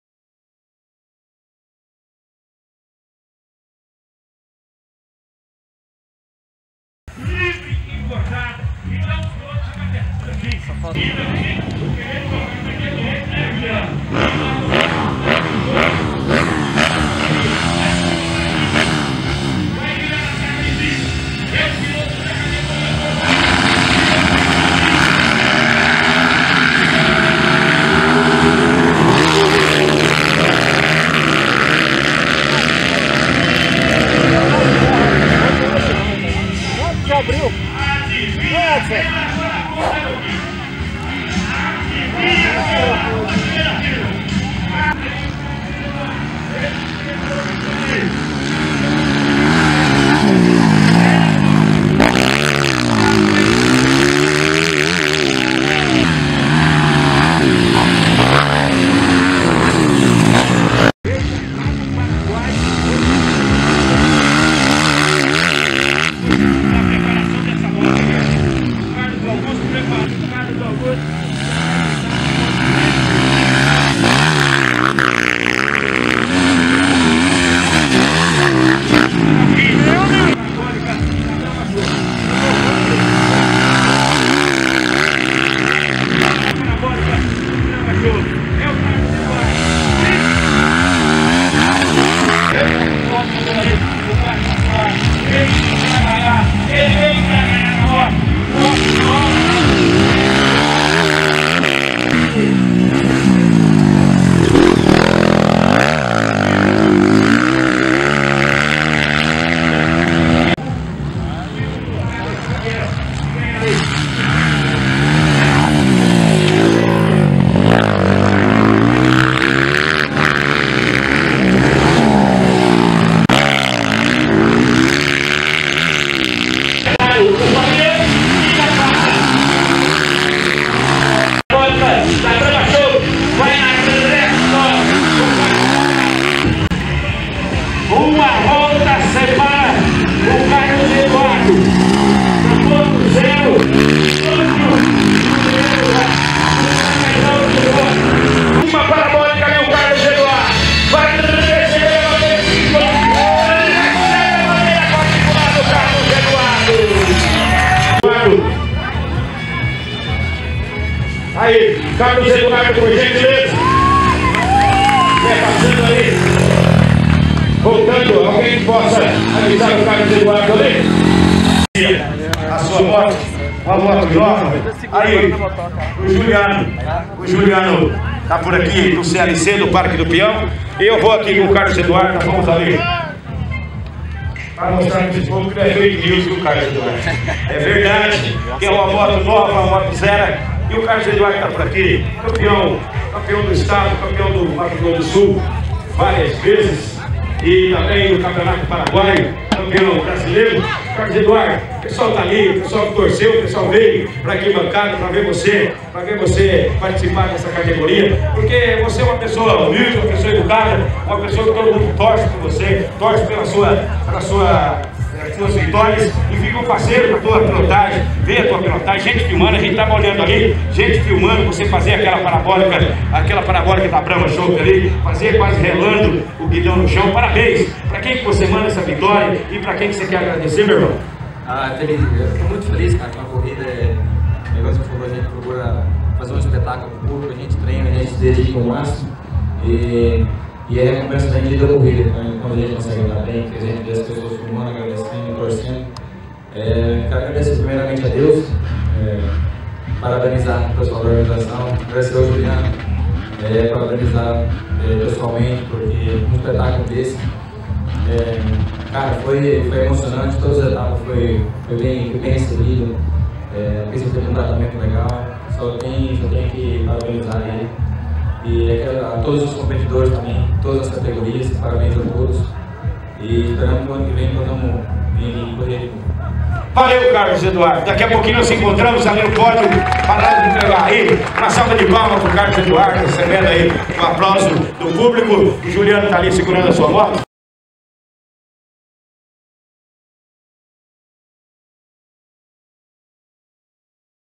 Vitória de Carlos Eduardo na Livre Nacional - Final Liga V 2018
Vitória de Carlos Eduardo Mendes Franco pela Liga V de Velocross, na etapa final do certame, realizada em 09/12, na pista do CLC, no Parque do Peão, em Campo Grande (MS)

Foto Capa: Paulo Cezar/V12